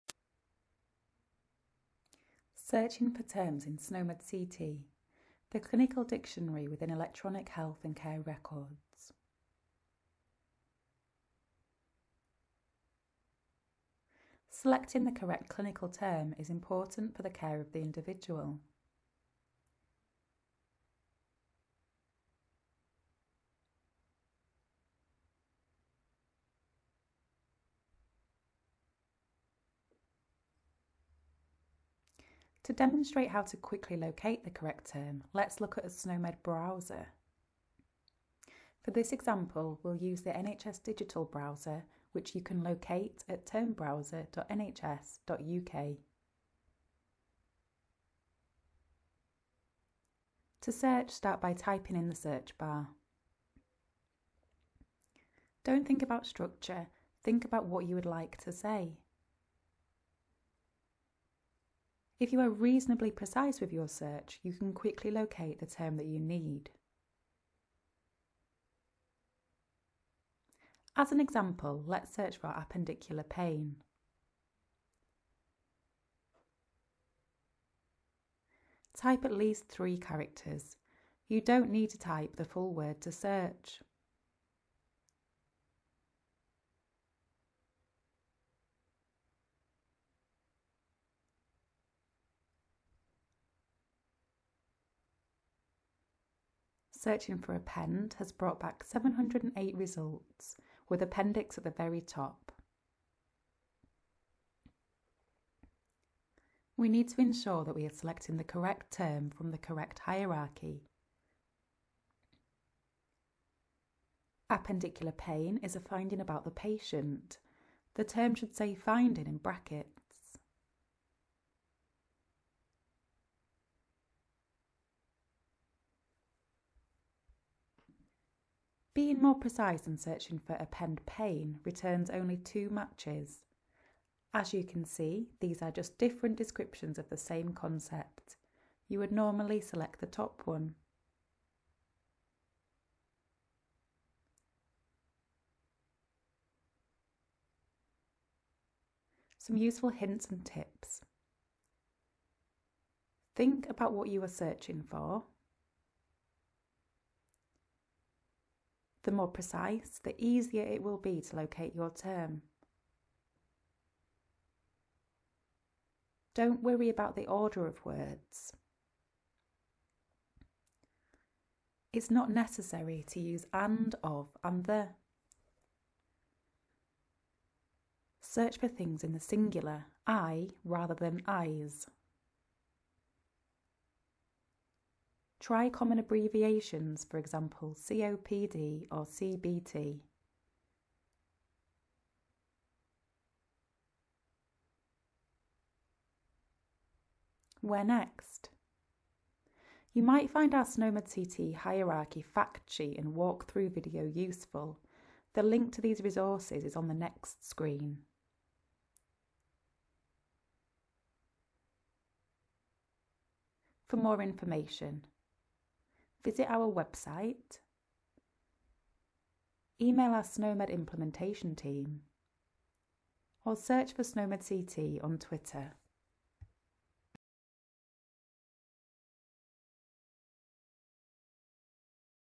SNOMED CT: Searching for terms | NHS Digital
This SNOMED CT tutorial is part of a series of tutorials, which you can view

This animation explains about searching for terms in SNOMED CT.

To find out more about SNOMED CT, visit the NHS Digital